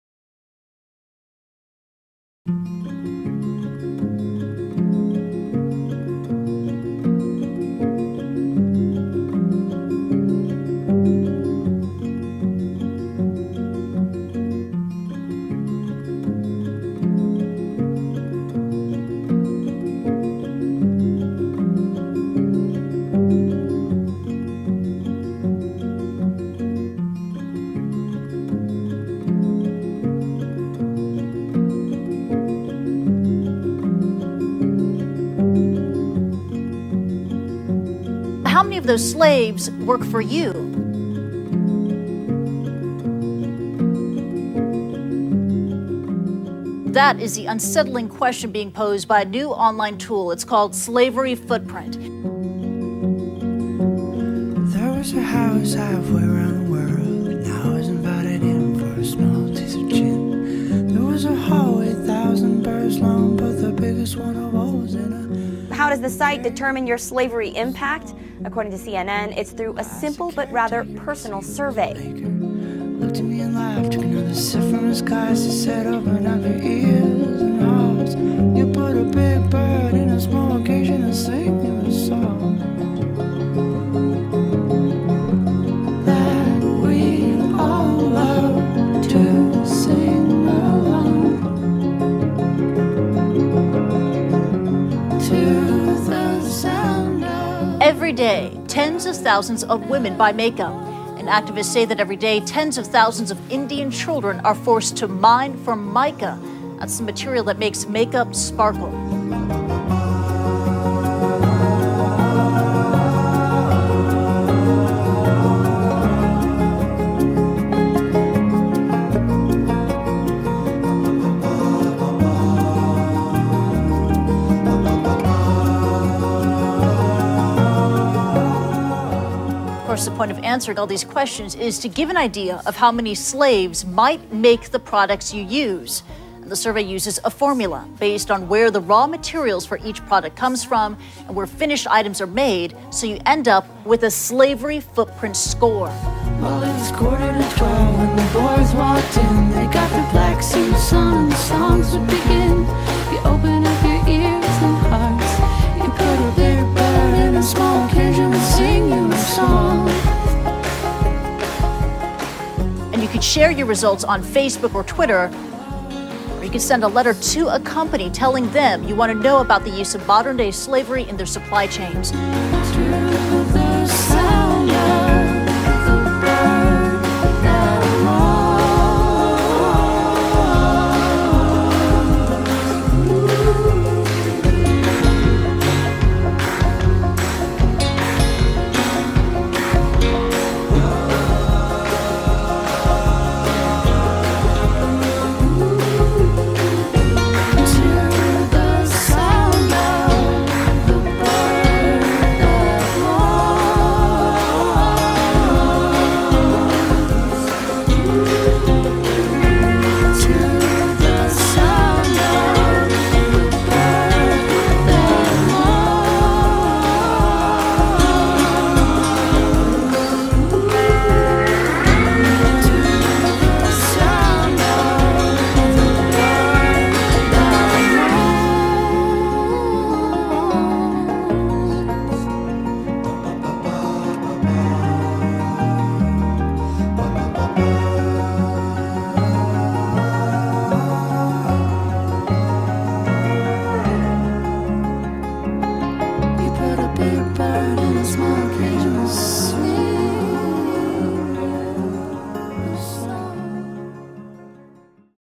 Made in a Free World - Slavery Footprint Case Study
There are more slaves bought and sold today than at any point in human history, but few people know. How do you take reams of State Department research and make it compelling and relevant? We asked a single question: "How many slaves work for you?"

Campaign goal: 150,000 surveys completed in a year.

Actual result: 1,000,000 surveys completed in the first month.
40,000 shares, 100 countries. 60,000 million media impressions on every major media outlet including CNN, NYT, Washington Post, NPR, LeMonde, Guardian, and hundreds more.

Pick of the Day, Creativity. Site of the Day, awwwards, Featured in How, Contagious.